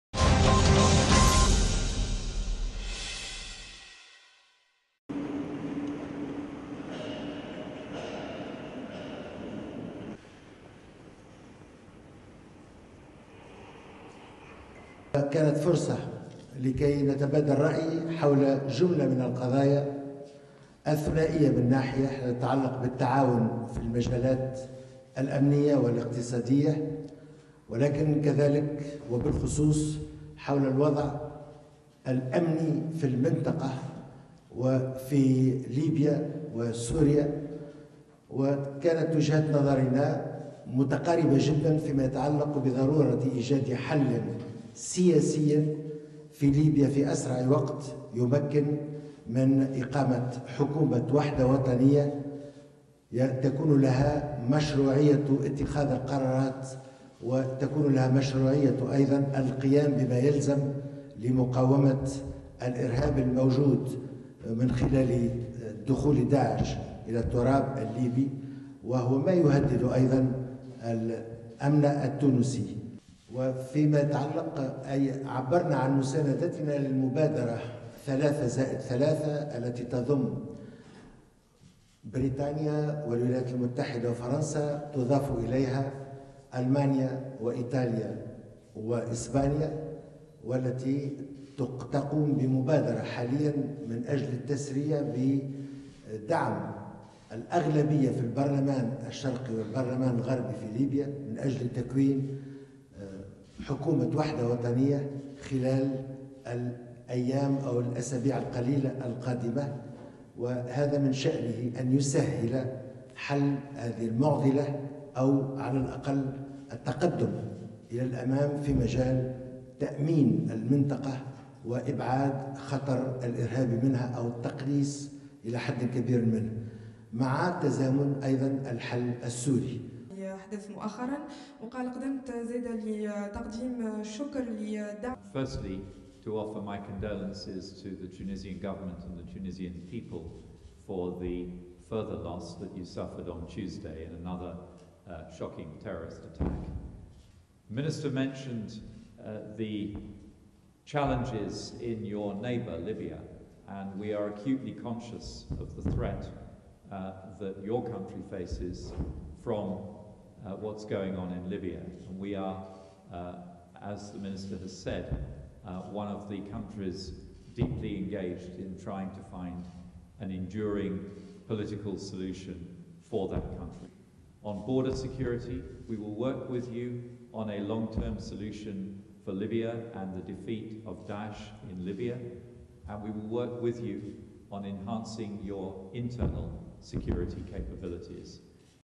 Philip Hammond Taieb Baccouche joint press conference
TUNIS, TUNISIA - NOVEMBER 27: British Foreign Secretary Philip Hammond and Minister of Foreign Affairs of Tunisia Taieb Baccouche hold a joint press conference in Tunis, Tunisia on November 27, 2015.